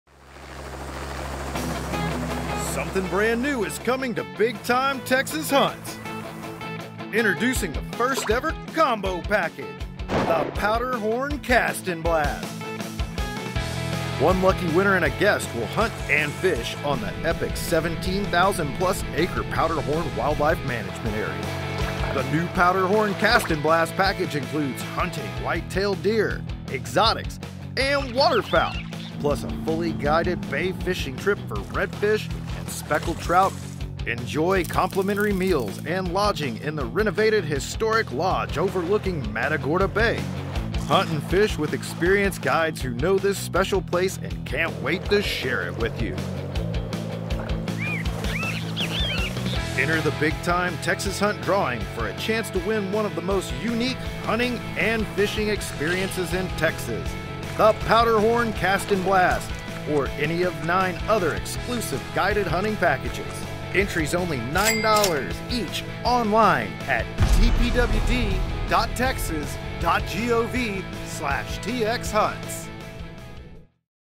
Powderhorn Cast and Blast!
This hunting and fishing extravaganza offers one lucky winner and a guest hunting for white-tailed deer, exotics and waterfowl on the epic 17,000+ acre Powderhorn WMA in Calhoun County  along with a guided fishing trip on Matagorda Bay.

Purchase by October 15, 2020

$9.00 per online entry*